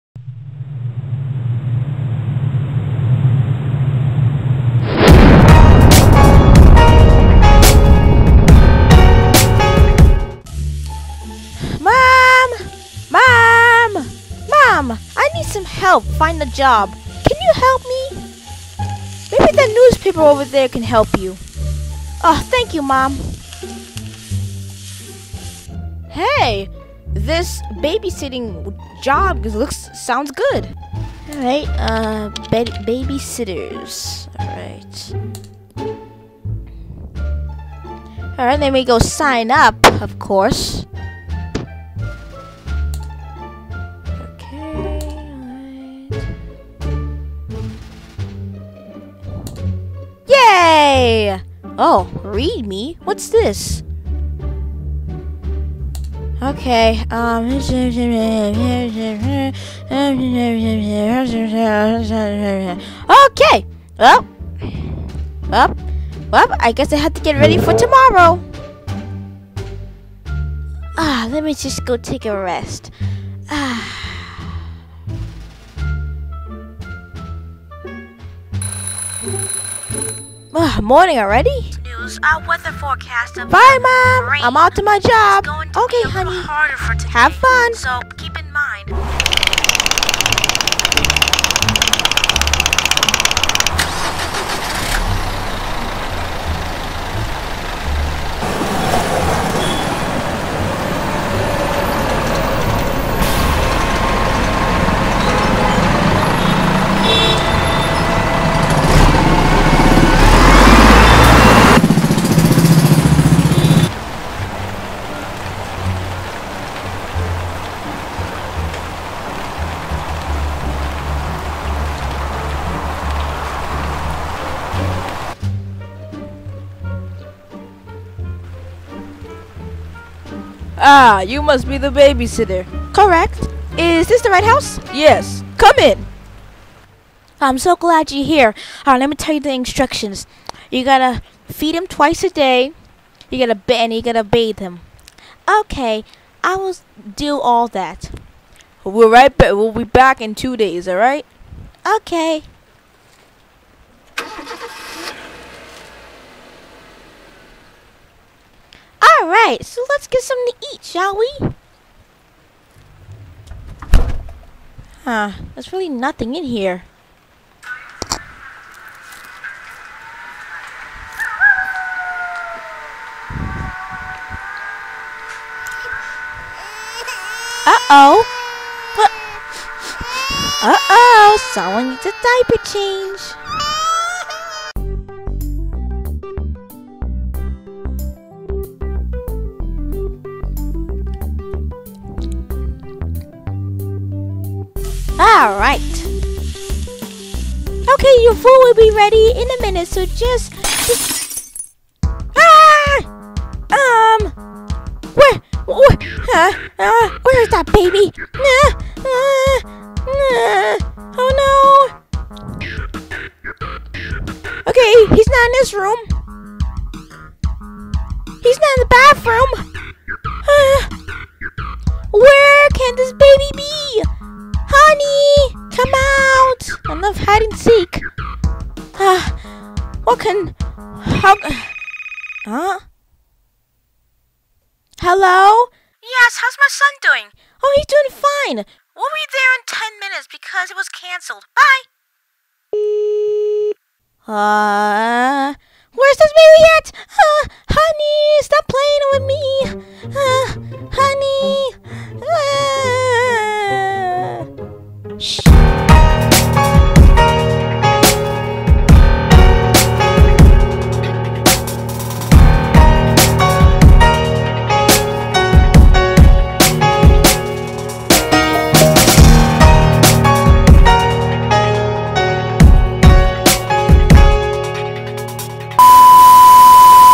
Lego Babysitter
Trying to find a job is really hard to do. But one girl found a job that would be good for. She found a babysitting job. I am 12 years old and I am still learning how to do Stop Motion. So please be kind, and enjoy the video. Thank You :)